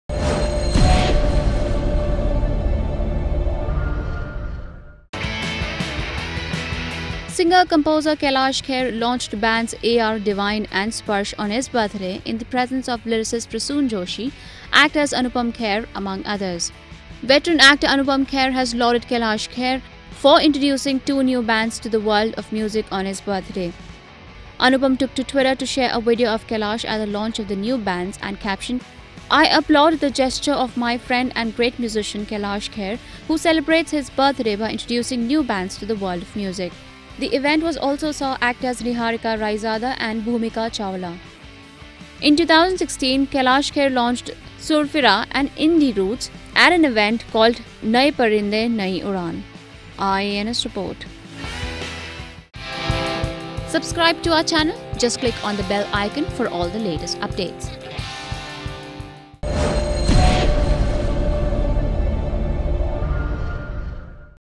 Anupam Kher lauds Kailash Kher for introducing New bands
Singer Composer Kailash Kher launched bands AR Divine and Sparsh on his birthday in the presence of lyricist Prasoon Joshi, actors Anupam Kher among others.  Veteran actor Anupam Kher has lauded Kailash Kher for introducing two new bands to the world of music on his birthday.